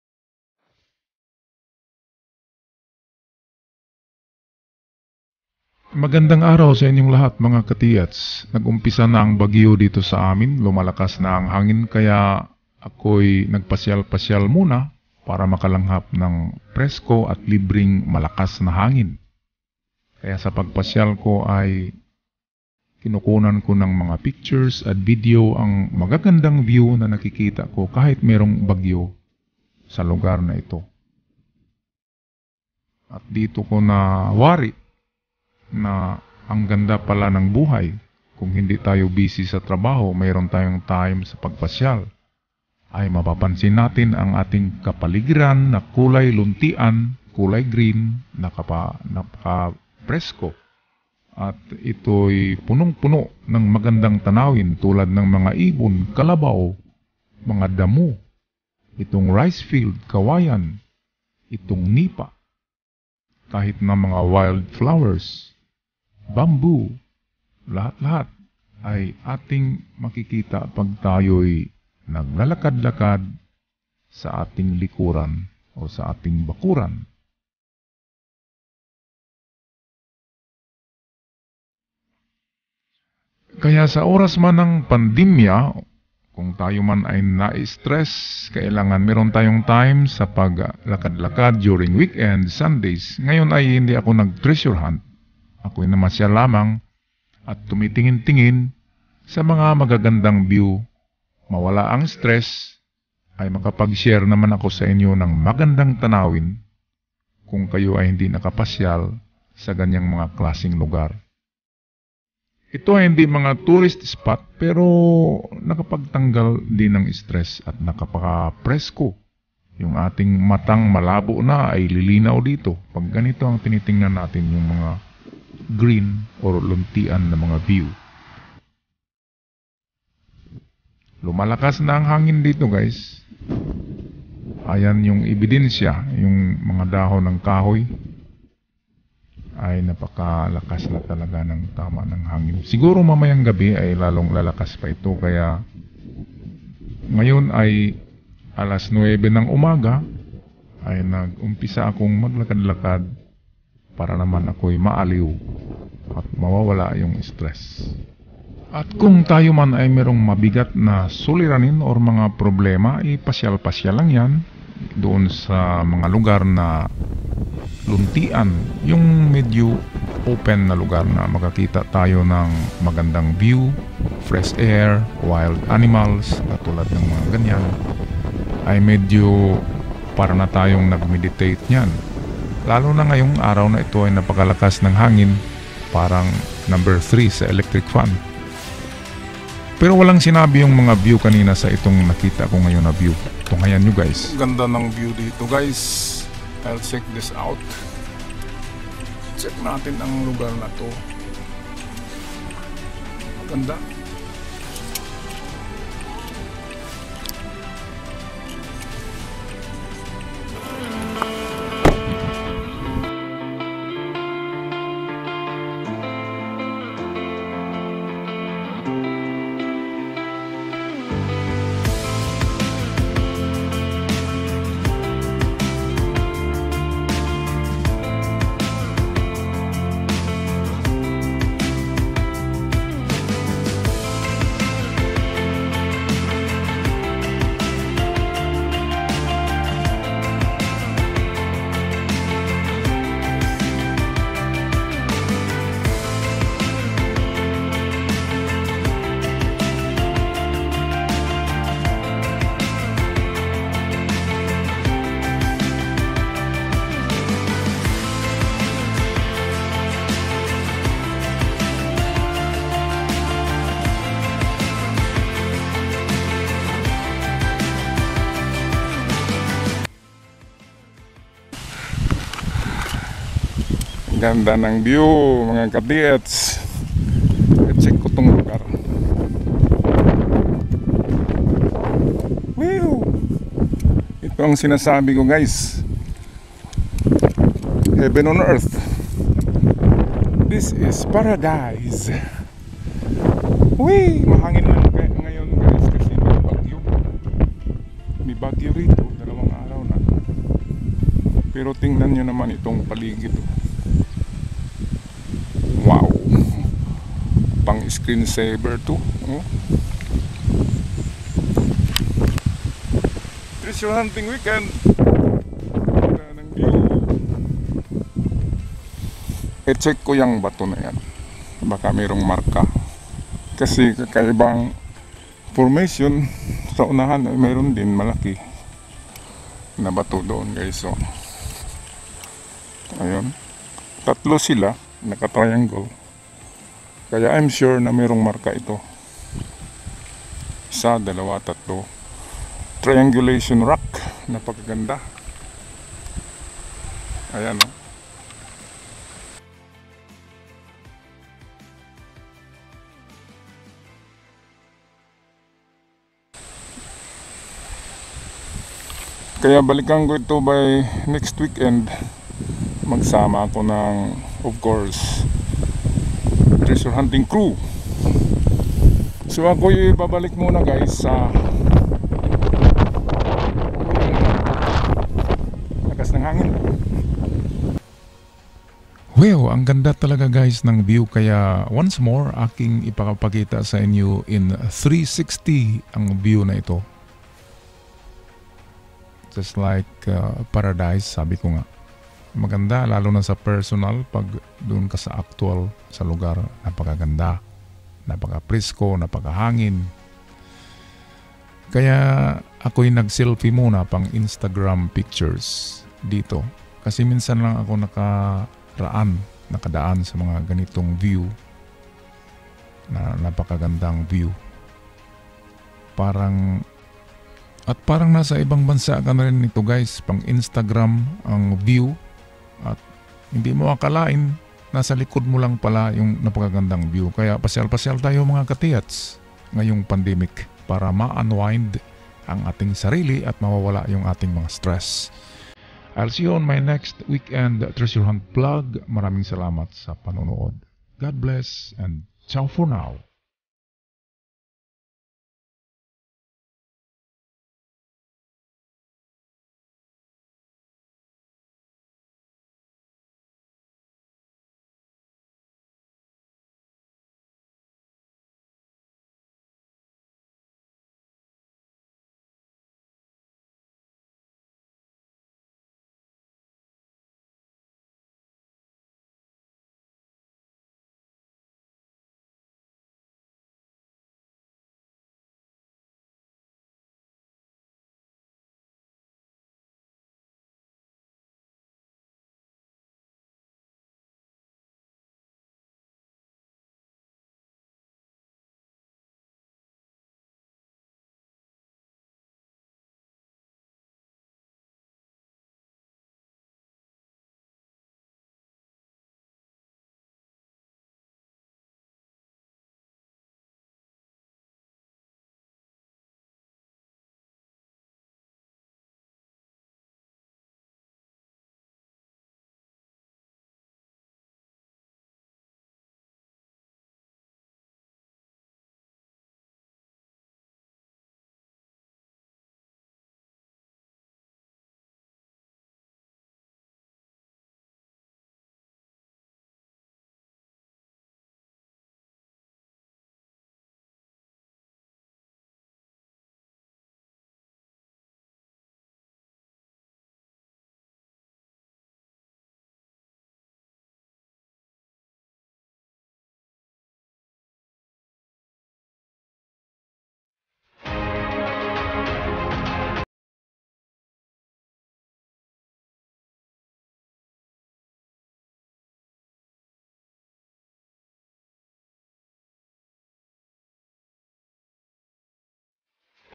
KAKAIBANG WEEKEND ITO MGA KA TH!!!
BUMABAGYO DITO NGAYON PERO I STILL SNEAK OUT TO SHOOT A VIDEO PARA SA INYO MG KA -TH PANG REFRESH.